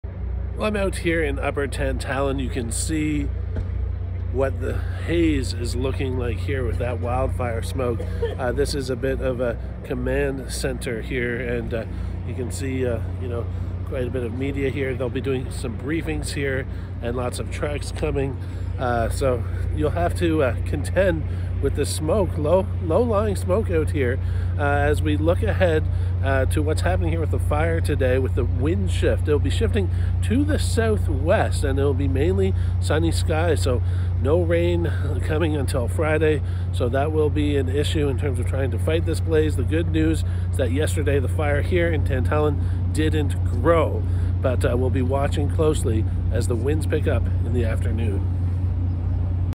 Smoky skies in Nova Scotia
Low-lying smoke creates hazy skies for Nova Scotia. The Weather Network's Nathan Coleman has more on wildfire conditions from Upper Tantallon, Nova Scotia.

 /weathernetwork